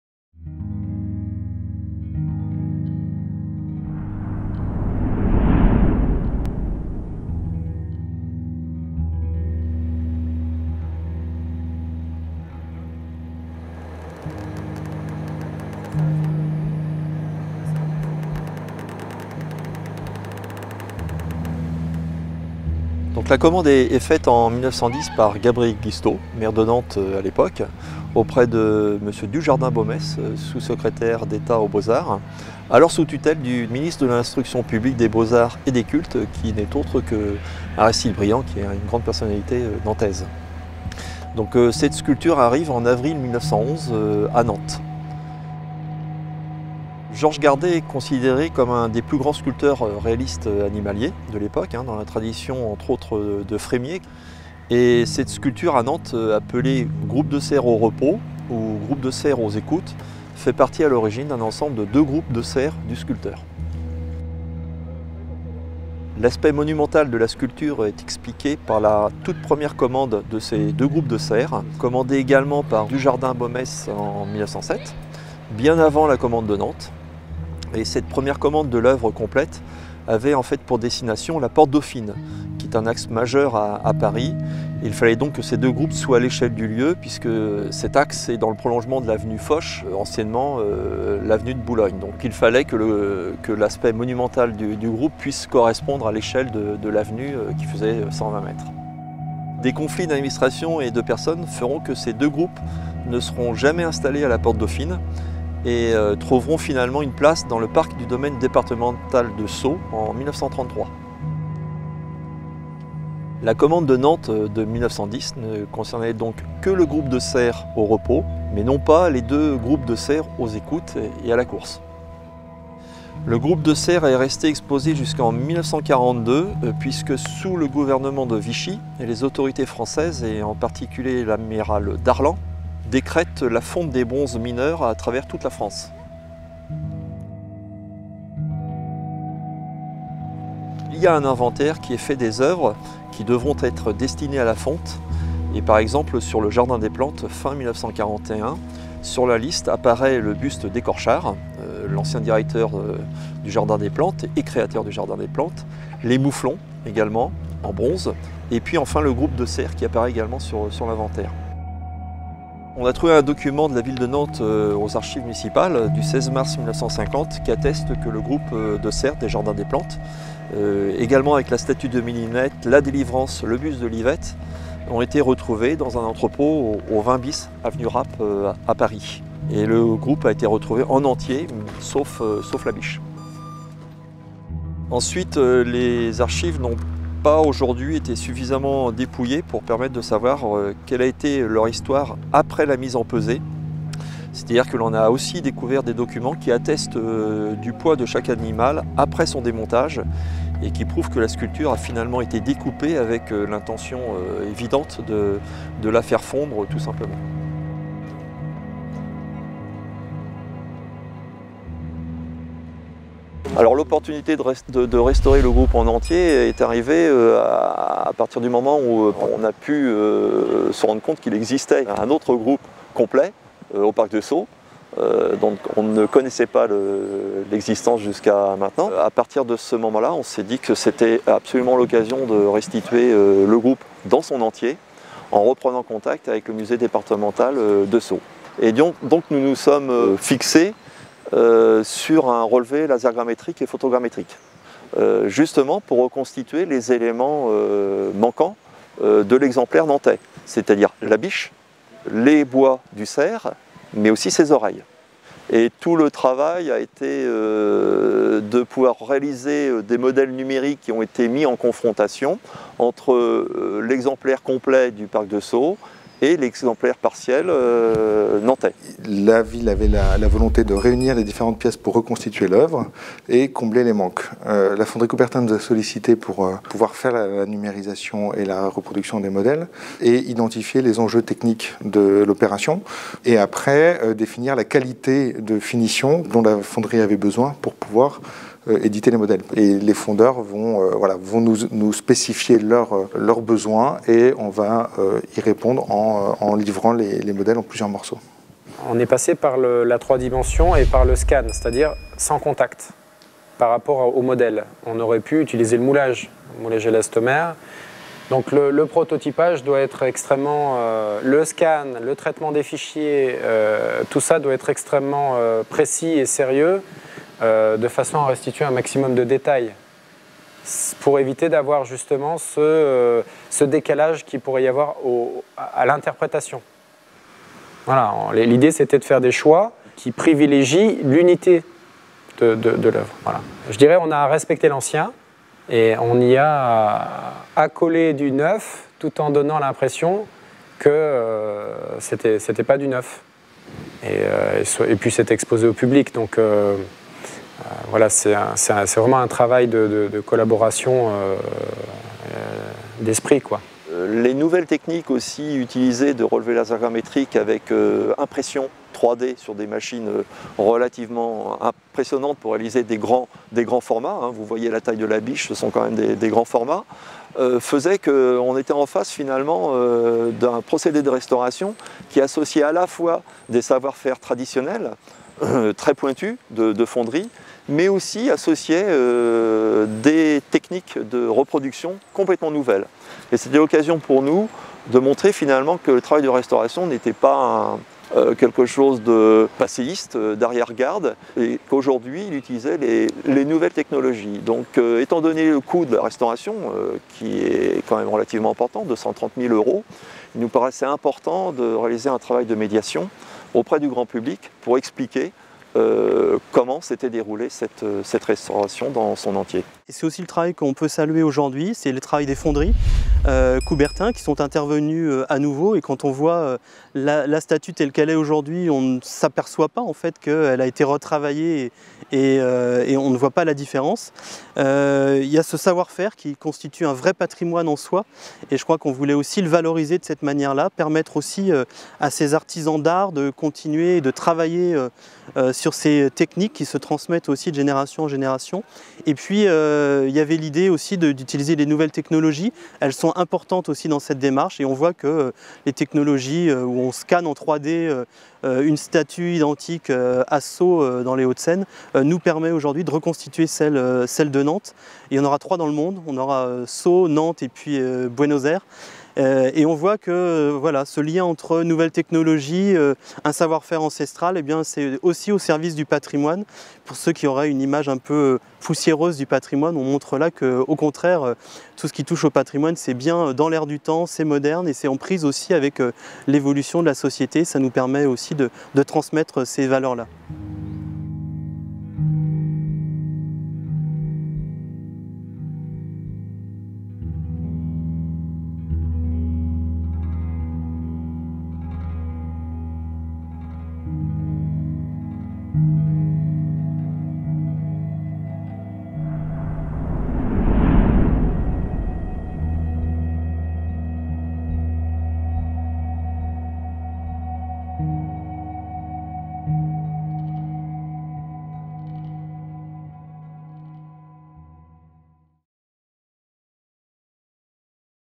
Reportage : Restauration de la sculpture des "Cerfs" de Georges Gardet du Jardin des Plantes | 2018
Avril 1911, arrivée de la sculpture "Groupe de cerfs au repos" au Jardin des Plantes de Nantes.
Pour en savoir plus sur la restauration, rendez-vous sur l'article Nantes Patrimonia